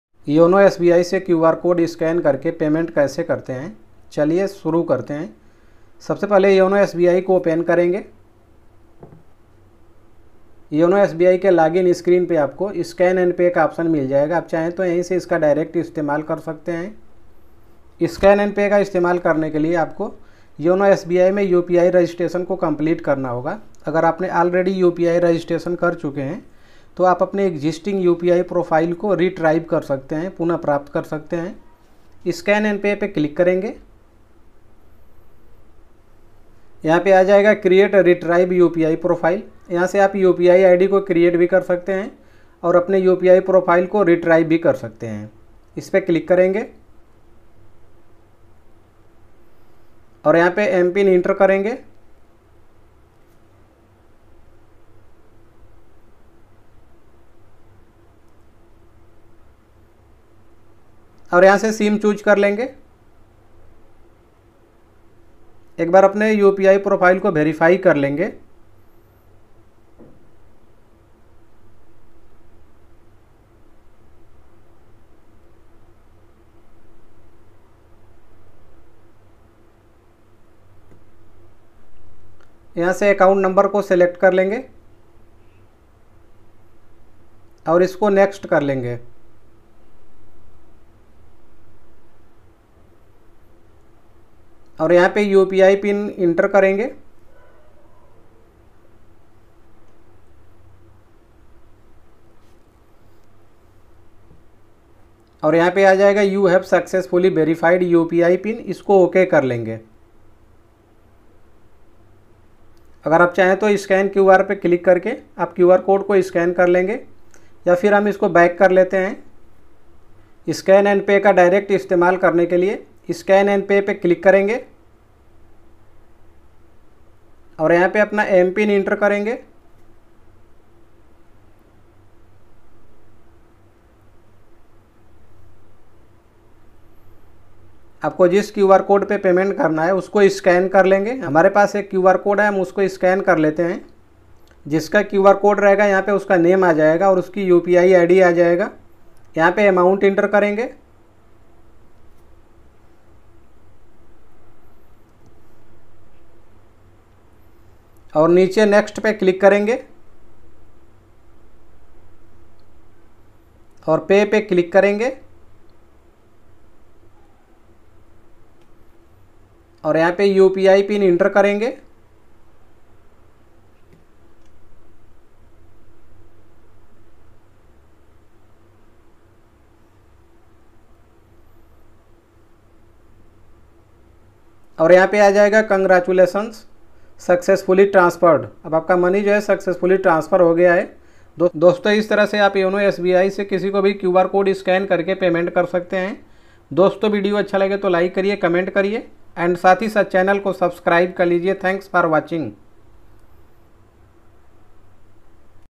Yono SBI से QR code scan करके payment कैसे करें | yono SBI scan QR payment kaise kare
yono SBI se QR code scan karke payment kaise karte hai video me complete process ko bataya gaya hai

yono SBI scan QR payment kaise kare
Yono SBI scan QR payment